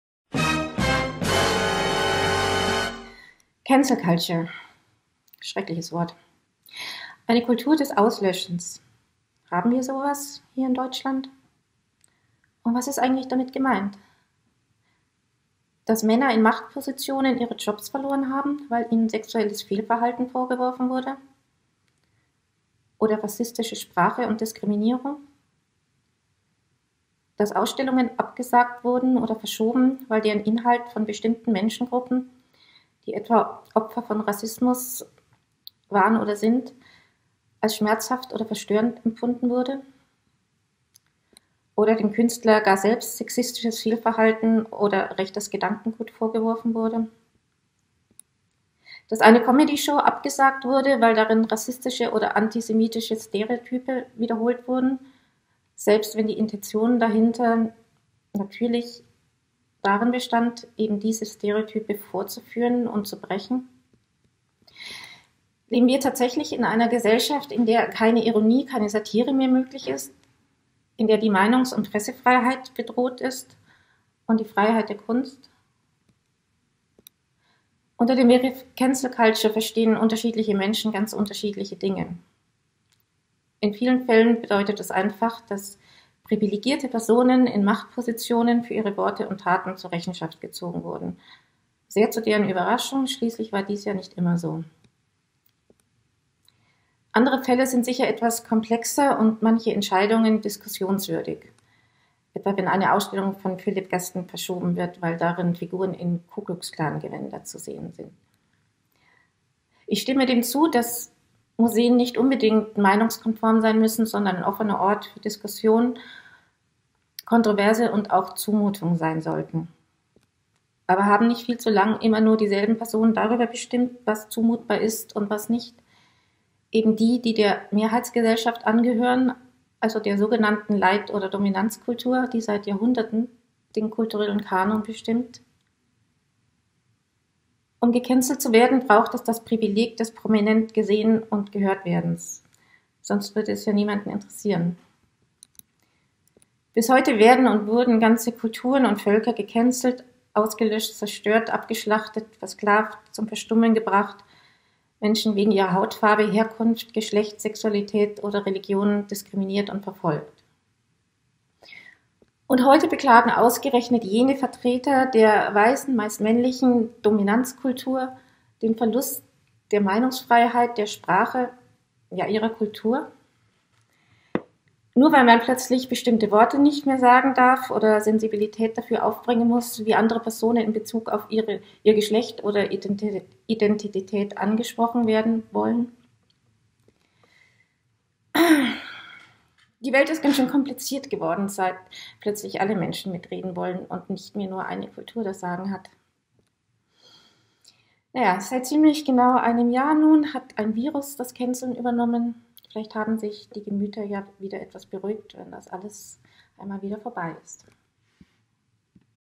TiG 03 / "Cancel Culture" / Eva Scharrer
TiG-03 / "Cancel Culture"
Language: German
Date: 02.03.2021

TiGGER:
Eva Scharrer (She/Her)
Writer + Curator, Berlin
Vintage: 1971

TRIGGER
‚Cancel Culture‘ bezeichnet das Boykottieren von Personen, Organisationen, Veranstaltungen oder Präsentationen, denen beleidigende oder diskriminierende, „politisch unkorrekte“ Aussagen bzw. Handlungen, oder in ihnen zum Ausdruck kommende Haltungen, vorgeworfen werden.

Die einen finden, der Auftritt oder die Präsentation an sich sei bereits aktiver Bestandteil des Machtdiskurses und sollte daher schon unterbunden werden; die anderen meinen, ein Diskurs könne überhaupt erst mit bzw. nach einem Auftritt oder einer Präsentation entstehen und geführt werden. Kann ‚Cancel Culture‘ zu einer „besseren“, „gerechteren“ Gesellschaft beitragen? Oder handelt es sich eher um die Unterdrückung und Diskreditierung bestimmter Meinungen und ihrer Vertreter*innen sowie die Debatten darum ohne wirkliche Legitimation? Wo endet die Forderung und der Anspruch auf Diversität? Obwohl der Begriff ‚Cancel Culture‘ eher für das linksintellektuelle Spektrum verwendet wird, findet das Phänomen ebenso im rechten Spektrum statt: etwa in Forderungen aus dem AfD-Lager, bestimmte Kulturszenen auszutrocknen, oder in der Kulturpolitik der aktuellen Regierungen in Polen, Ungarn, der Türkei oder China. In beiden Lagern sind die Forderungen nach Boykott und Absage meist von einem gefühlten Anspruch moralischer Überlegenheit durchdrungen.

Zahlreiche Fälle haben in den letzten drei, vier Jahren zu heftigen Debatten und mittlerweile auch zu etlichen Entlassungen geführt. Das Spektrum reicht von der me-too-Debatte (Woody Allen-Biografie, Jon Rafman etc.) über Auftritte von Performern aus dem rechten Spektrum (Axel Krause/Leipziger Jahressausstellung, Lisa Eckhart etc.) bis zu Absagen in vorauseilendem Gehorsam aus Furcht vor vermeintlich unkontrollierbaren Debatten (Feine Sahne Fischfilet/Dessau, Philip Guston/Tate Modern) und hat auch den akademischen Bereich in den Universitäten längst erreicht („Netzwerk Wissenschaftsfreiheit“).

TiG-03 wird mit der großzügigen Unterstützung des Sprengel Museum Hannovers präsentiert. Die Folge wird von Reinhard Spieler (Direktor, Sprengel Museum Hannover) und Candice Breitz kuratiert.

BEHIND THE SCENES:
Episode Concept: Candice Breitz + Reinhard Spieler
Above Text: Reinhard Spieler
Design + Sound: Alex Fahl
Production: Alex Fahl